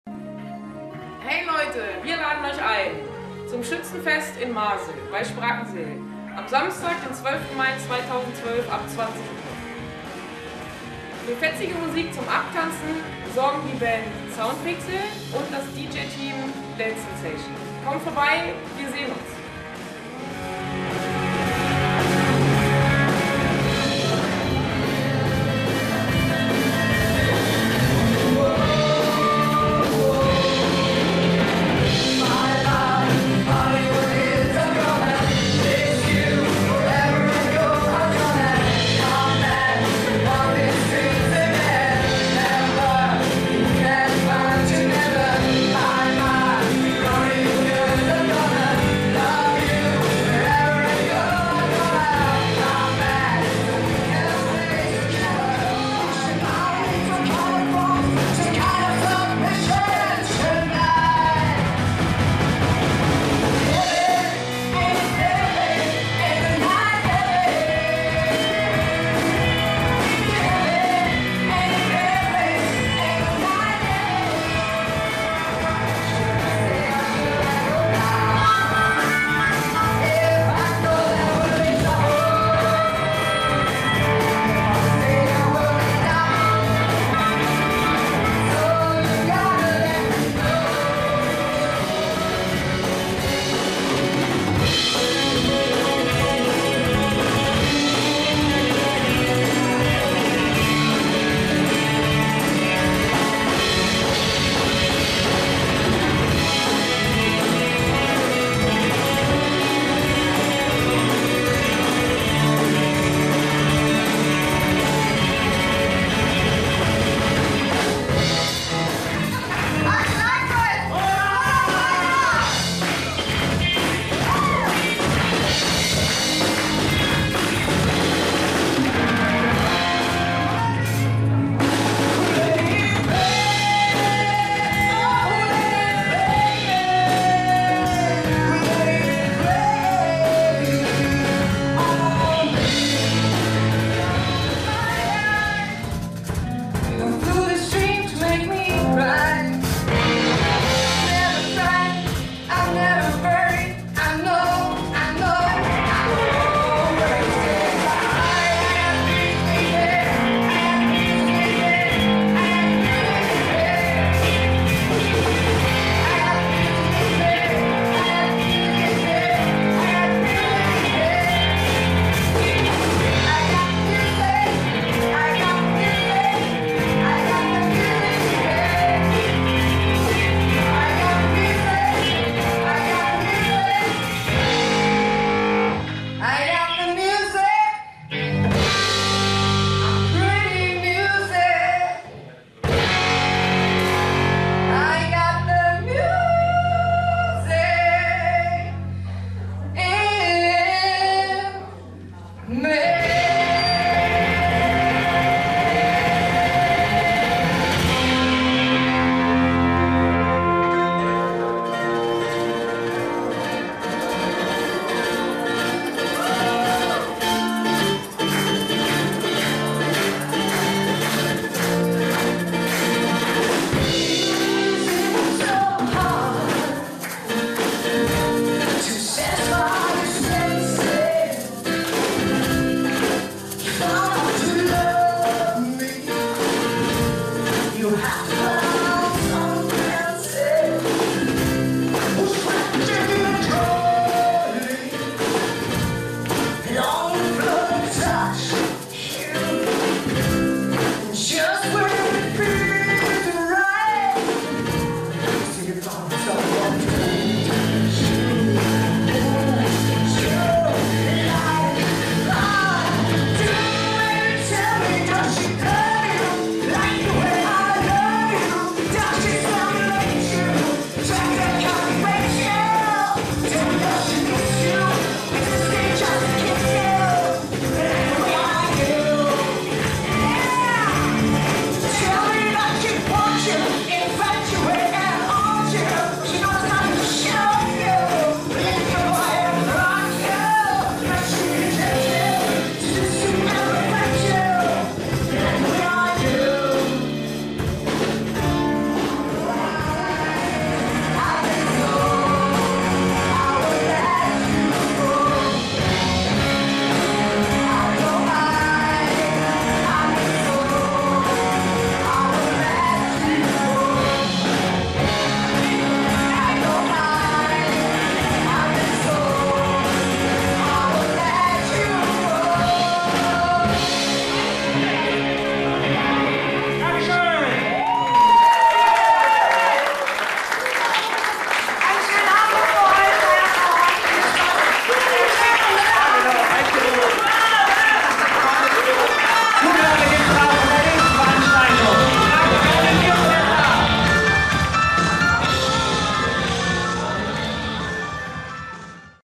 Schützenfest Masel 2012
Trailer zum Schützenfest in Masel (Sprakensehl) am 12. Mai 2012 mit der Band Soundpixel und dem DJ-Team Dance Sensation